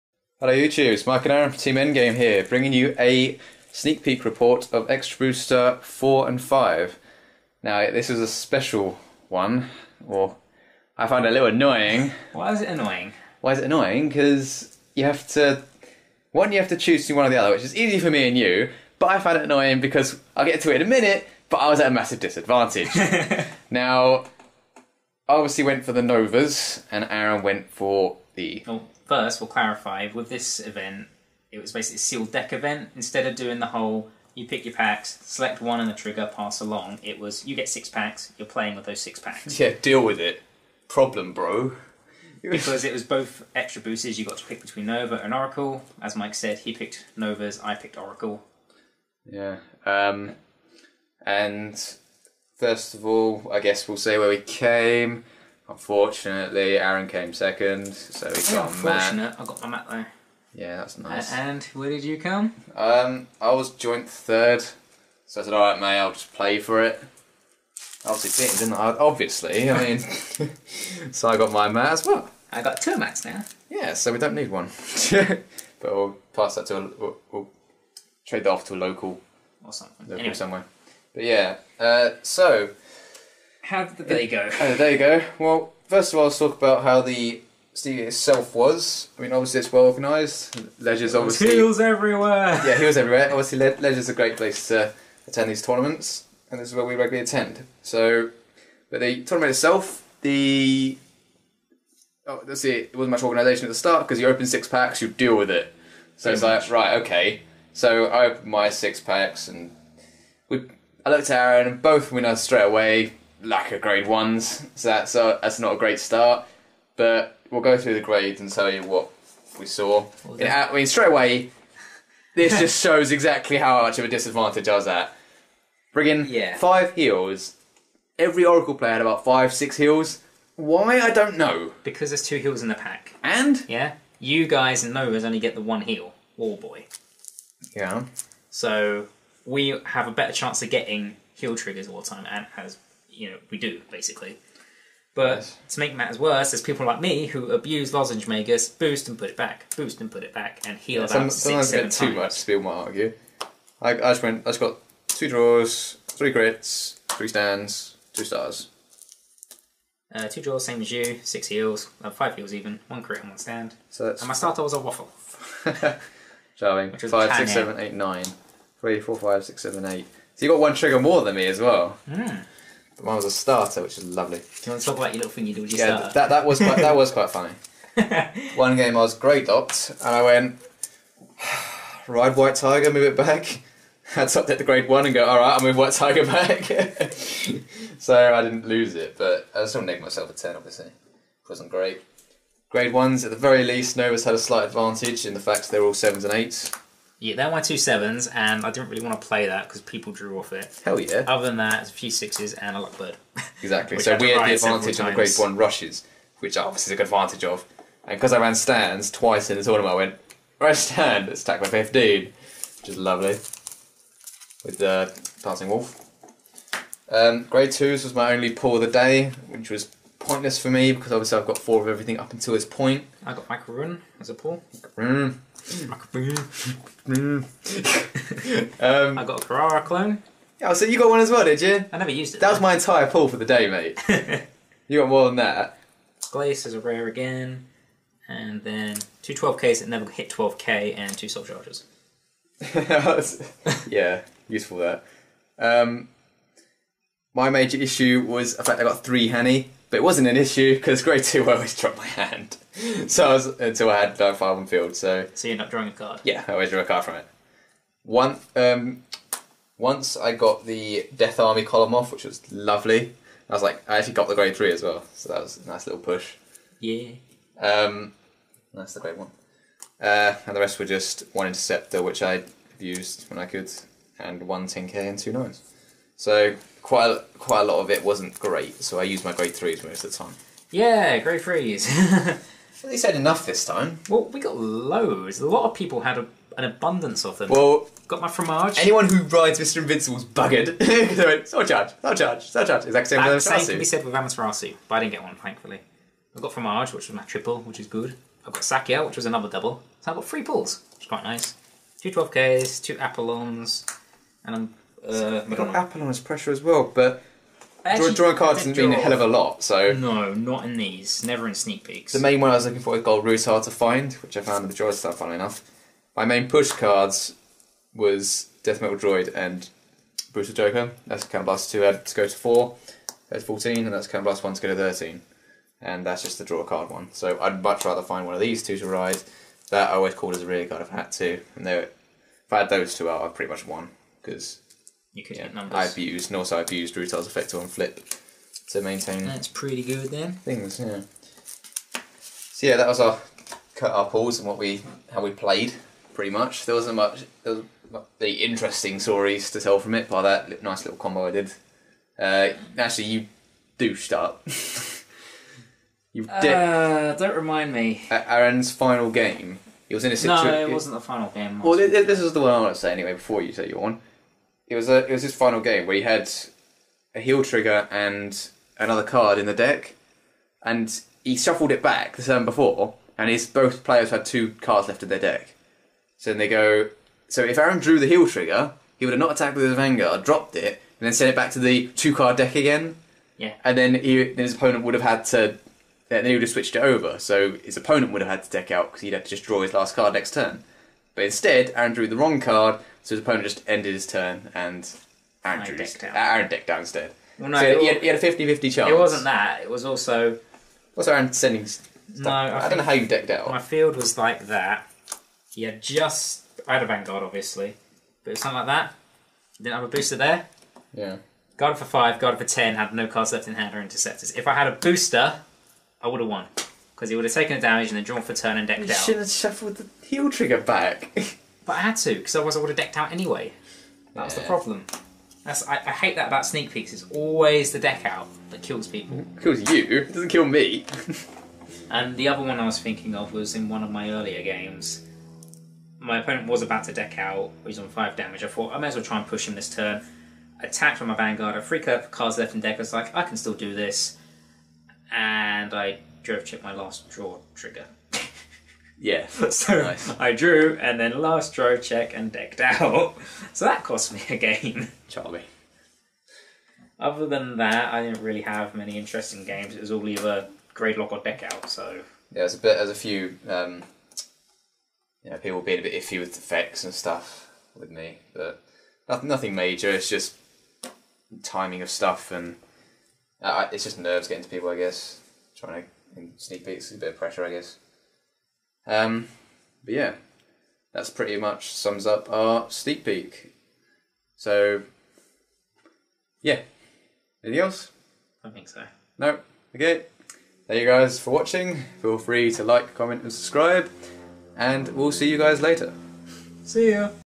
Cardfight!! Vanguard - EB04/05 Infinite Phantom Legion/Celestial Valkyries Sneak Preview Report
Our EB04/05 Tournament Report.